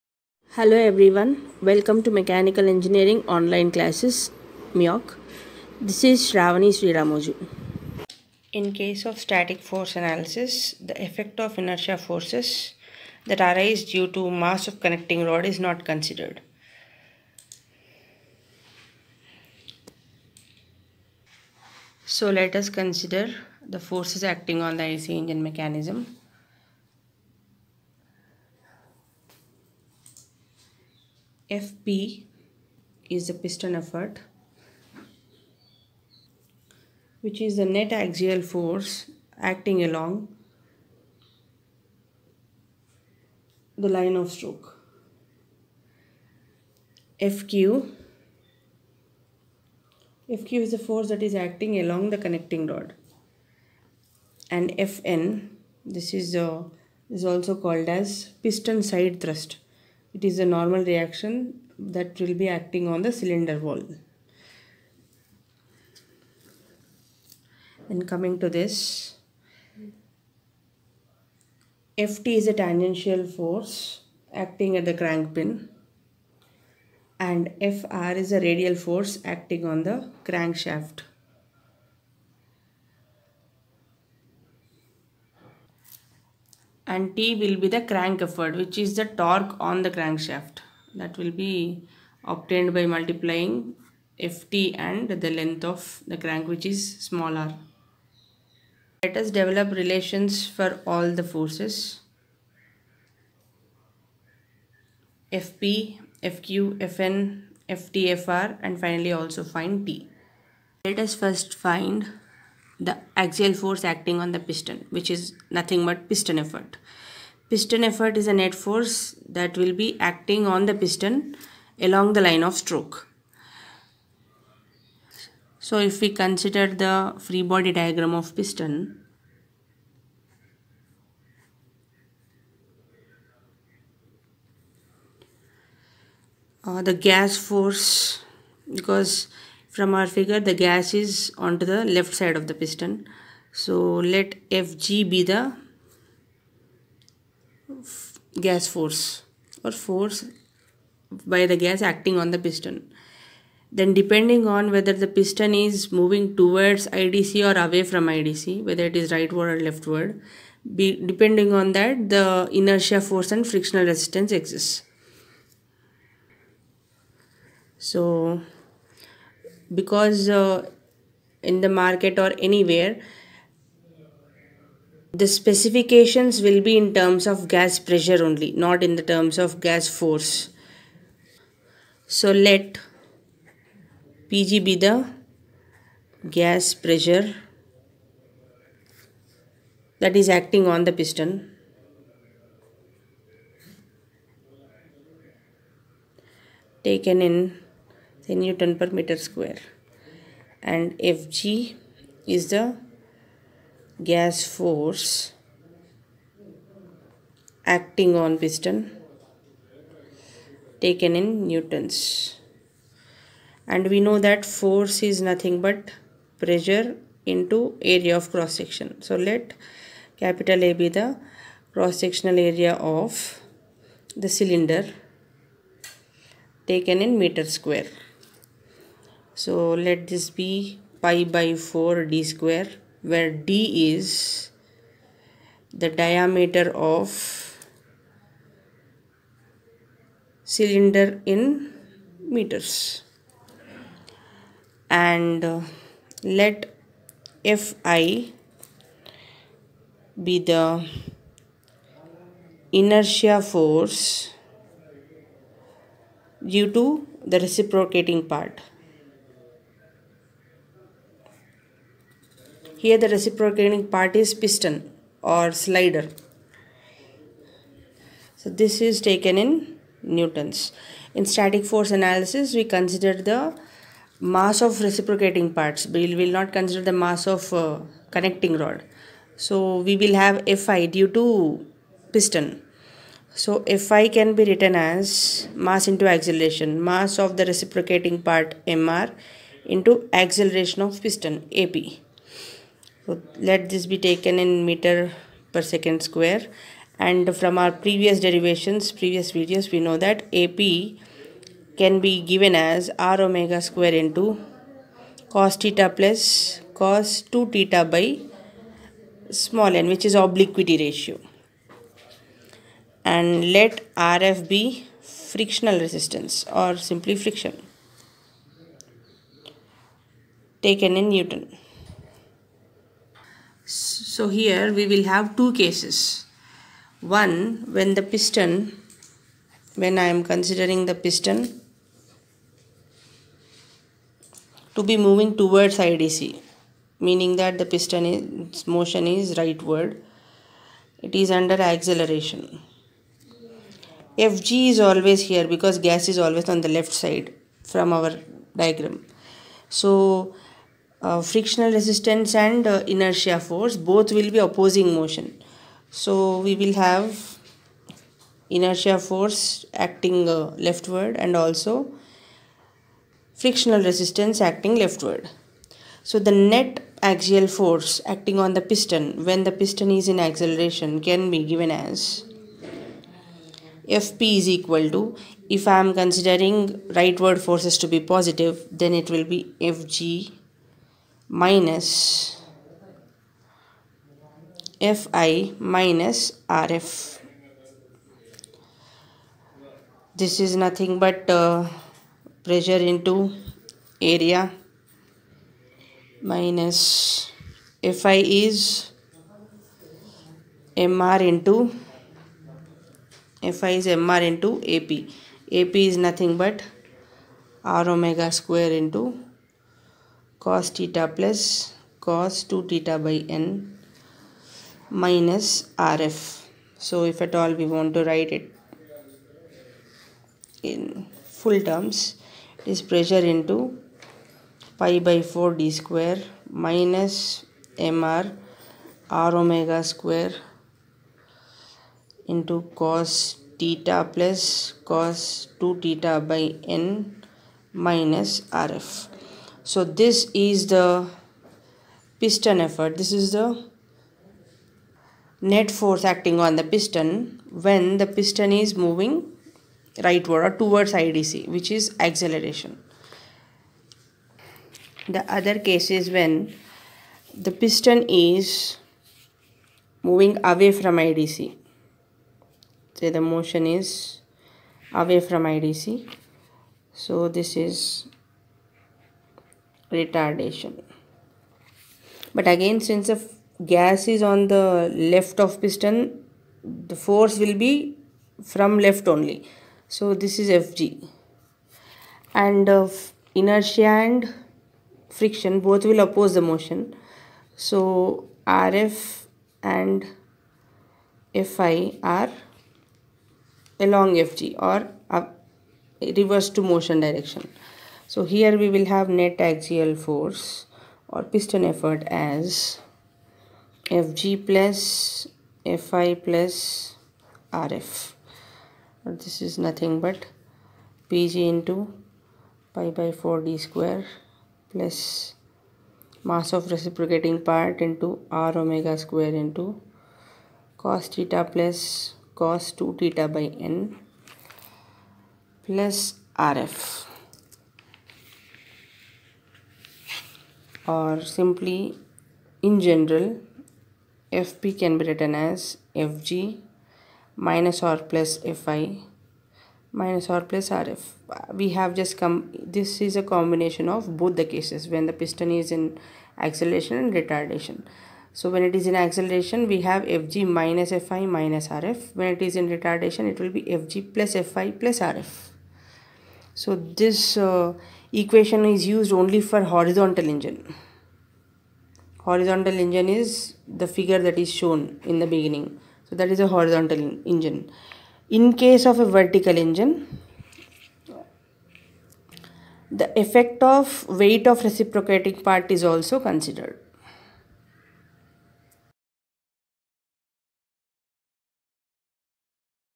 Piston effort in slider crank mechanism ||Static Force Analysis || IC Engine mechanism
Static force analysis of slider crank mechanism in which net axial force on the piston along line of stroke is derived for a horizontal engine

** Usage of images, videos, sounds without permission may invite legal troubles